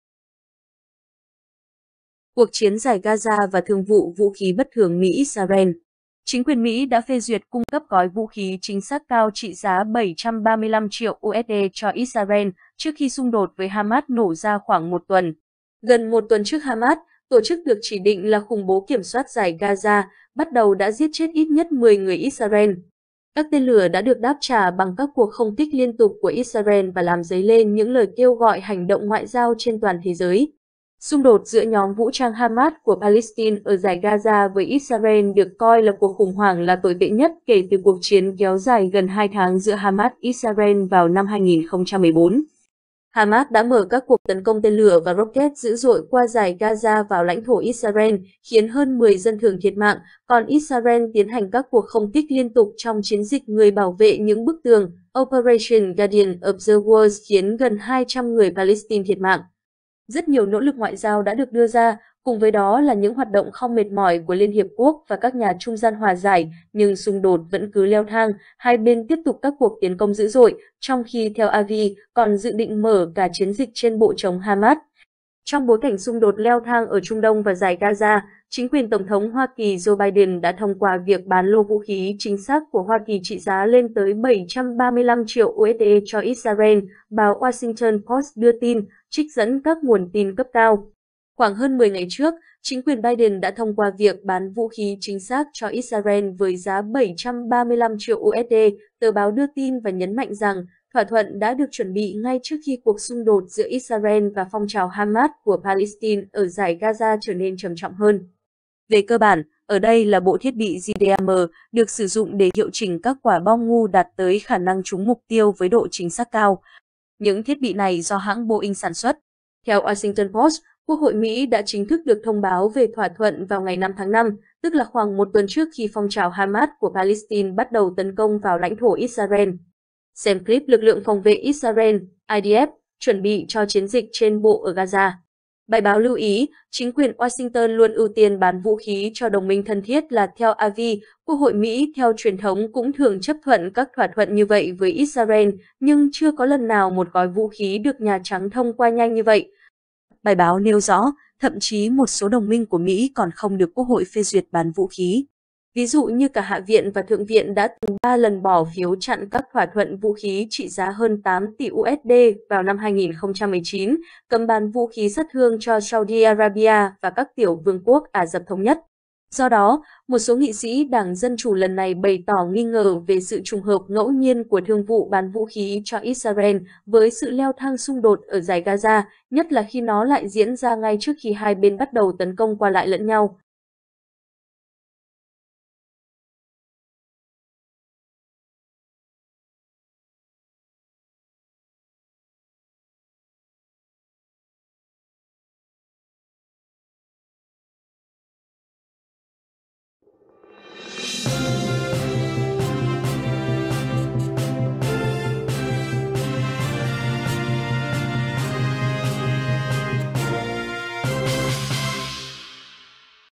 Cuộc chiến dải Gaza và thương vụ vũ khí bất thường Mỹ-Israel
Trong bối cảnh xung đột leo thang ở Trung Đông và dải Gaza, chính quyền Tổng thống Hoa Kỳ Joe Biden đã thông qua việc bán lô vũ khí chính xác của Hoa Kỳ trị giá lên tới 735 triệu USD cho Israel, báo Washington Post đưa tin, trích dẫn các nguồn tin cấp cao.

"Khoảng hơn 10 ngày trước, chính quyền Biden đã thông qua việc bán vũ khí chính xác cho Israel với giá 735 triệu USD" - tờ báo đưa tin và nhấn mạnh rằng, thỏa thuận đã được chuẩn bị ngay trước khi cuộc xung đột giữa Israel và phong trào Hamas của Palestine ở dải Gaza trở nên trầm trọng hơn.

Về cơ bản, ở đây là bộ thiết bị JDAM, được sử dụng để hiệu chỉnh các quả bom ‘ngu’ đạt tới khả năng trúng mục tiêu với độ chính xác cao. Những thiết bị này do hãng Boeing sản xuất.

Theo Washington Post, Quốc hội Mỹ đã chính thức được thông báo về thỏa thuận vào ngày 5 tháng 5, tức là khoảng một tuần trước khi phong trào Hamas của Palestine bắt đầu tấn công vào lãnh thổ Israel.